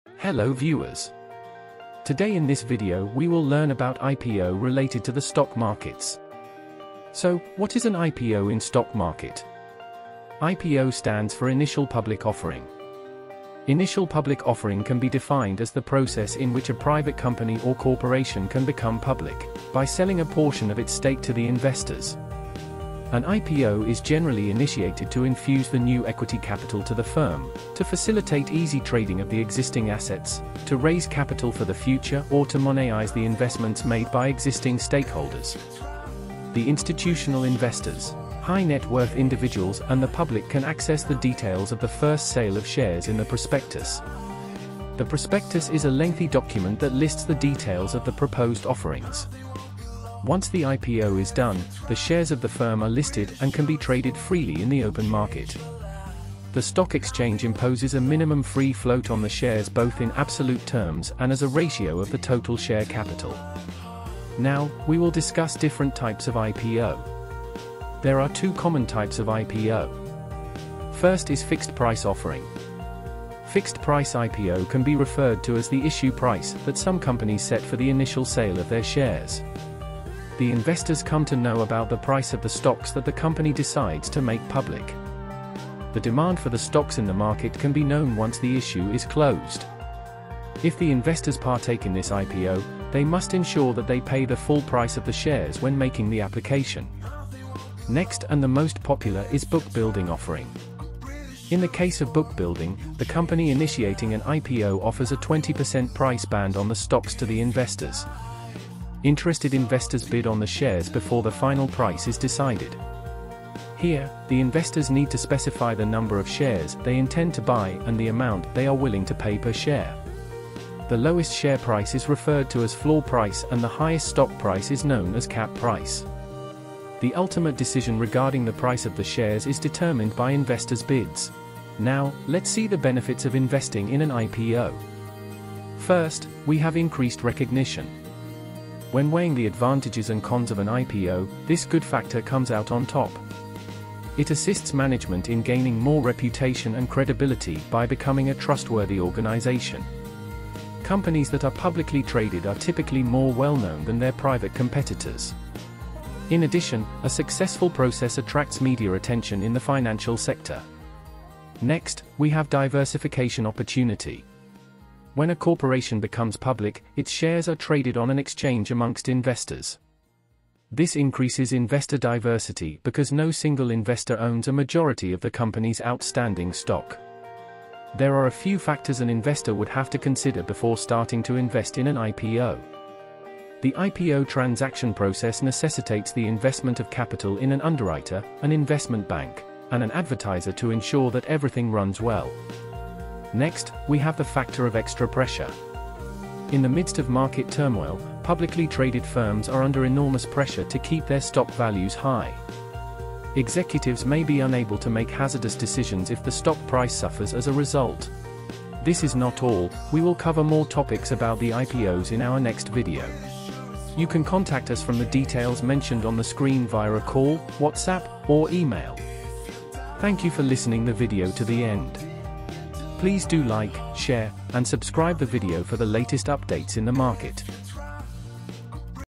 IPO in a Nutshell Part-1 | Types, Pros & Cons Explained!
It's a big deal when a stock IPOs, and you may be interested in participating, but it's important to understand the IPO process before jumping on the bandwagon, because as you'll see, you face some unique risks and disadvantages when buying a company's first shares.

The Initial Public Offering (IPO) market in India has emerged as a key player in the global financial landscape. An IPO is a process through which a privately held company raises funds by selling shares to the public for the first time.

In this part of the video, we will cover: -

0:00 INTRODUCTION
0:08 DEFINITION OF IPO
1:04 TYPES OF IPO
2:16 PROS AND CONS OF IPO